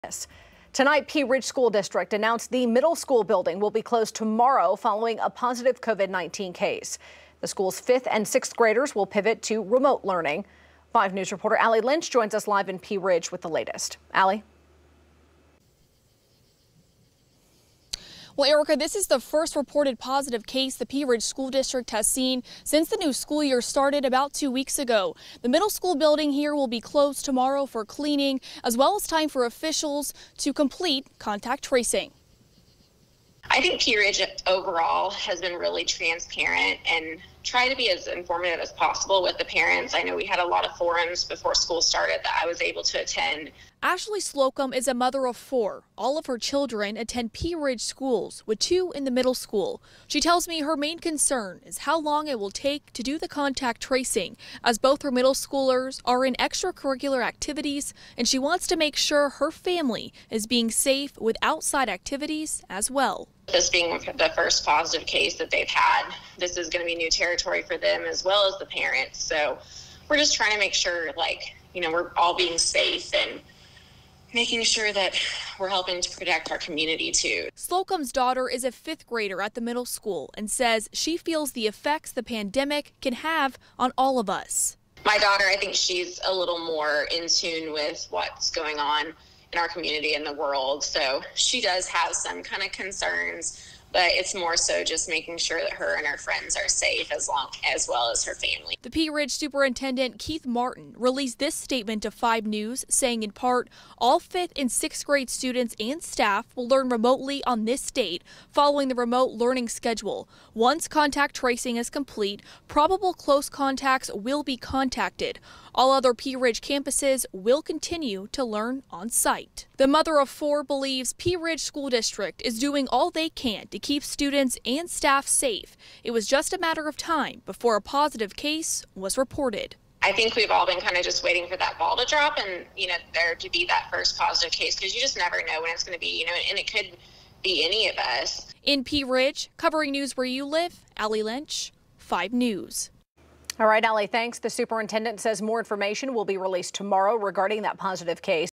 Pea Ridge Middle School Moves to Remote Learning for Wednesday Following Positive COVID-19 Case Repo
Pea Ridge Middle School is pivoting to remote learning for Sept. 9, following a positive COVID-19 case being reported in the building.